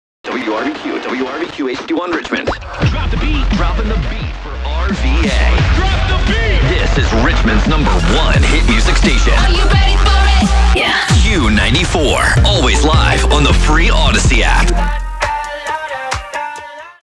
[Tropo] WRVQ "Q94" - Legal ID - 5am (7/31/2023)
Received In Raleigh, North Carolina.